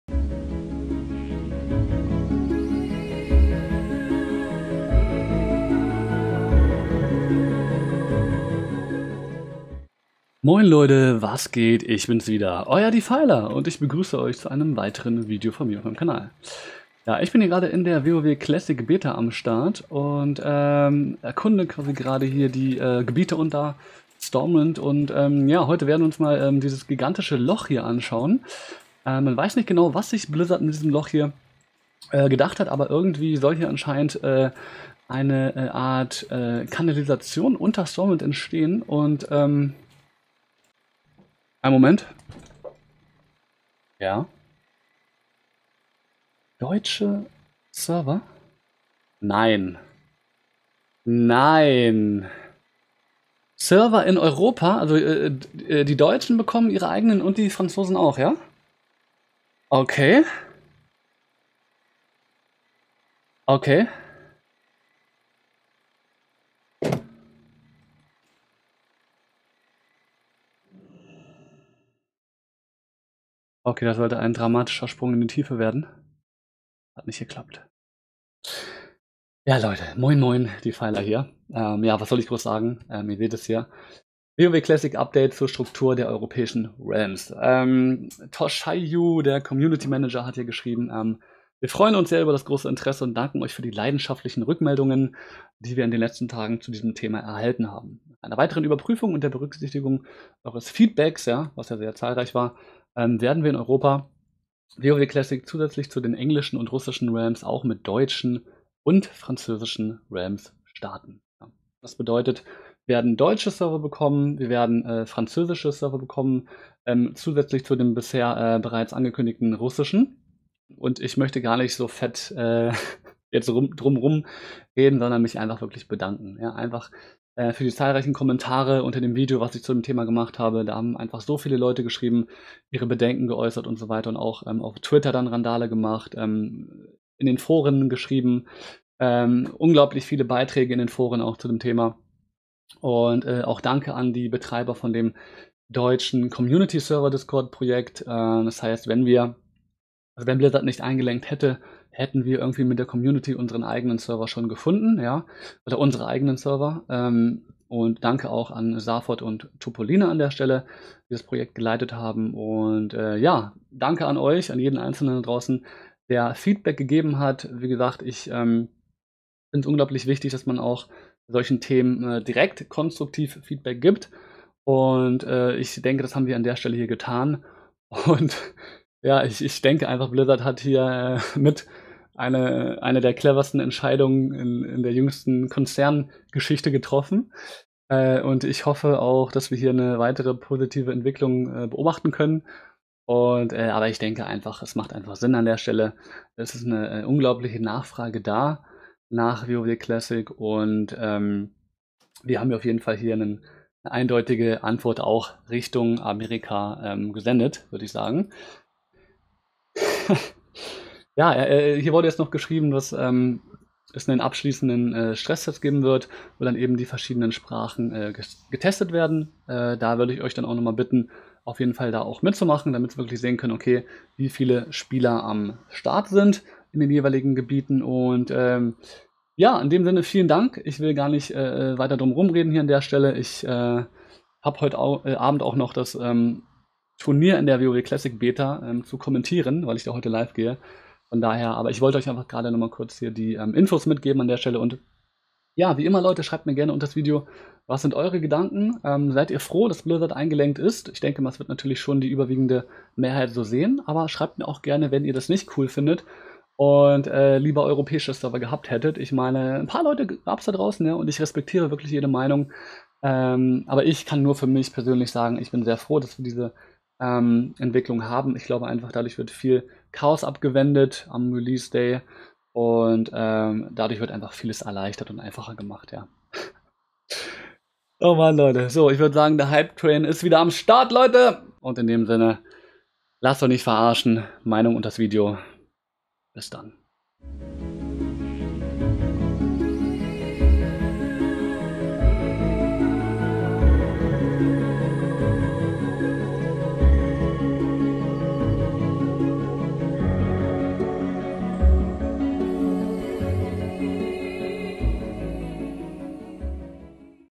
Nationale WoW Classic Server! 😍Die Community hat gesiegt! 🙏 | Defiler 😈
(Sonntag, Dienstag & Donnerstag ab 19 Uhr live!)

TeamNoLife - Befreundete Streamer

Es wird nun doch nationale WoW Classic Server geben! DANKE an euch alle! Danke an die europäischen Communities, die das mit ihrem konstruktiven Feedback ermöglicht haben!

LG Defiler